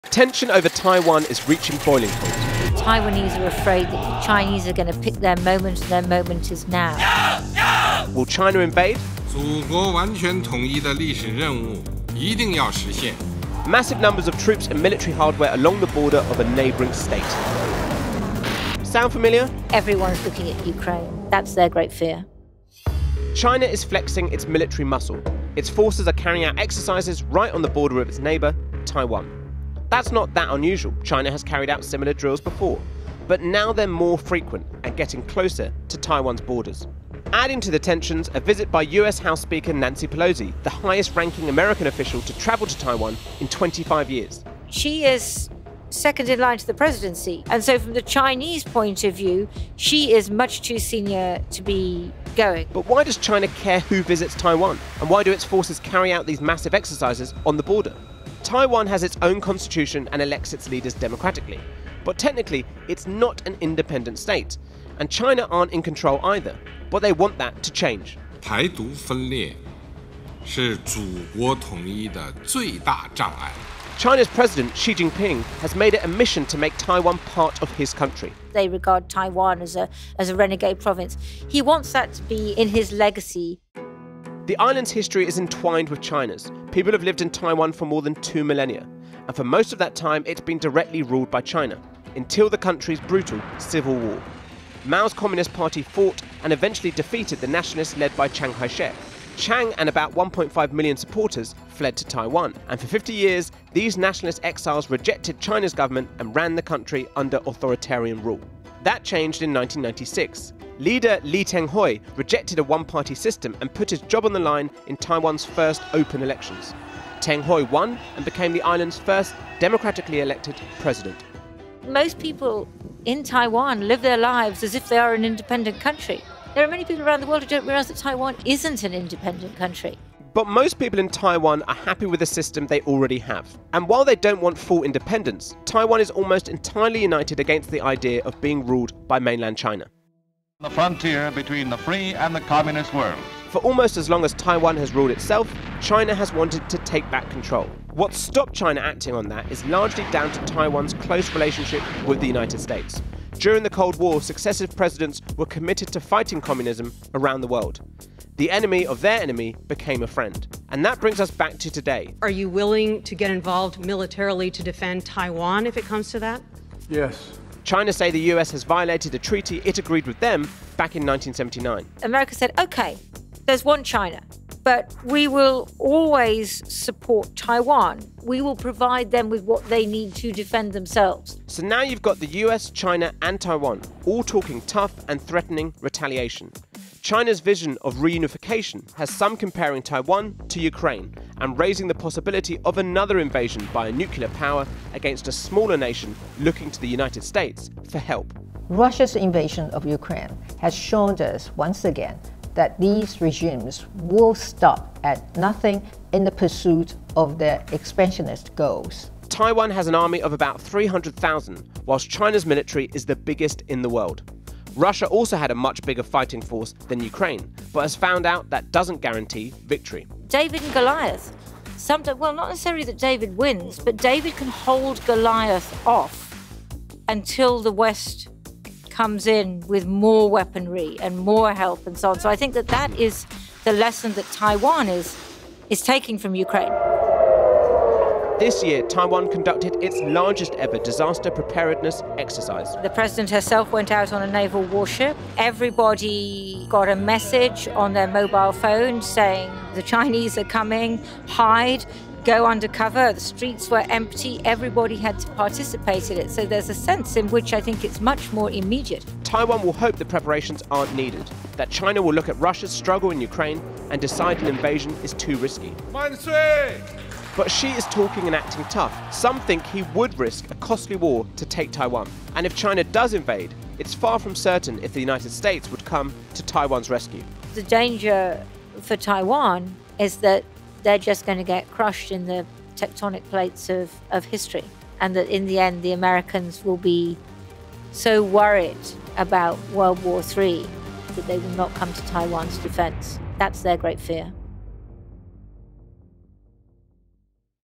Explained: Is Taiwan the next Ukraine?
Massive numbers of troops and military hardware massing along the border of a neighbouring state - sound familiar?

China is flexing its military muscle.  Its forces are carrying out exercises right on the border of Taiwan. That’s not that unusual - China has carried out similar drills before, but now they're more frequent and getting closer to Taiwan itself.

Is China is about to invade? And if it does, will the US come to Taiwan's aid, and could Taiwan be about to become the next Ukraine?

According to archaeological evidence, Taiwan was originally inhabited entirely by Austronesians who trace their cultural links to the islands to the south and east. When the Dutch set up their small colonial operations in southern Taiwan in the early 17th century, occupying a tiny area, there was little Chinese presence on the island.